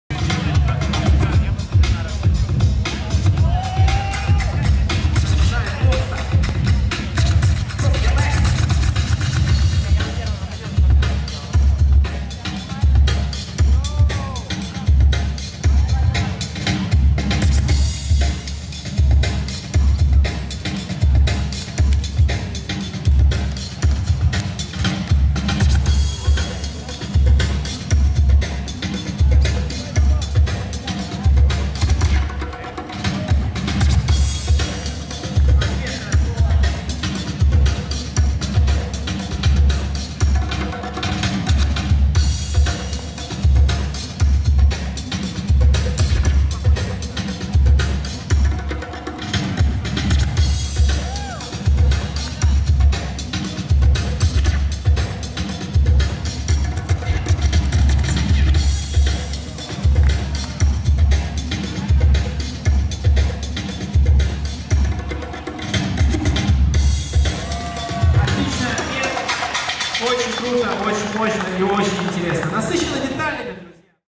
Osta vs Vorobei | Footwork Concrete 1/16 at FORMSKILL 2022
Breaking Festival in Russia - "Form Skill"